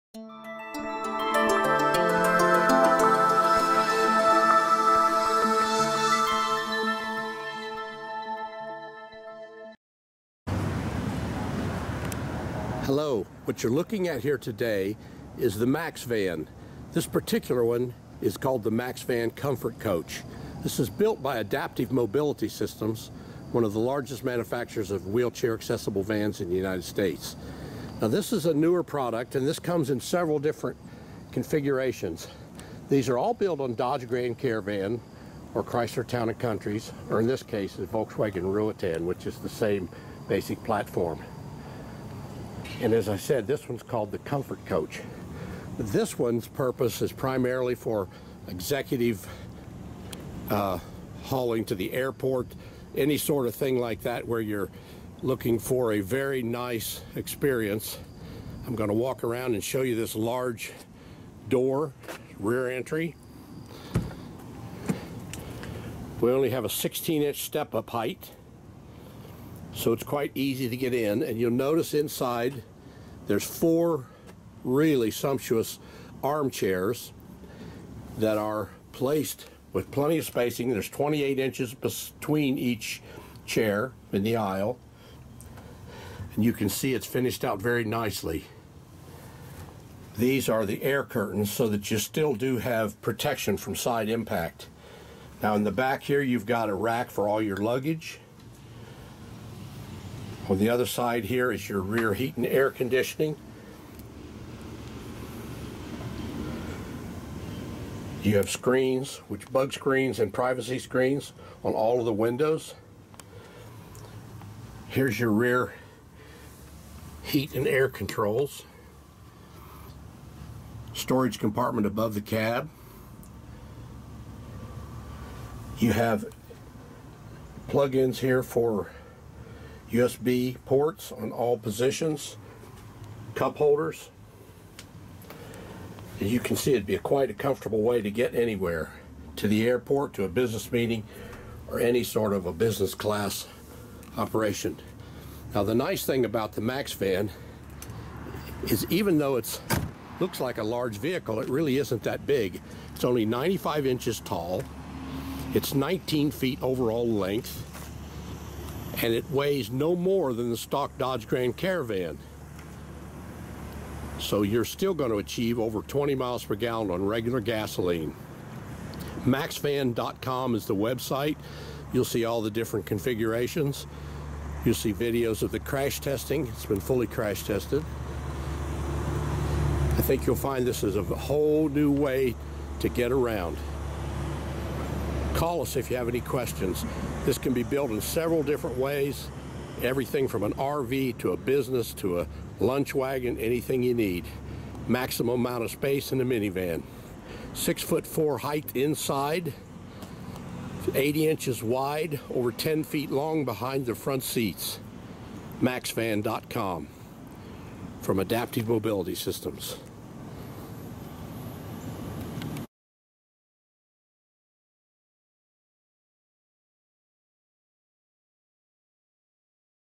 MaxVan Comfort Coach
MaxVan comfort coach built by Adaptive Mobility Systems walk around video. for more information on our products.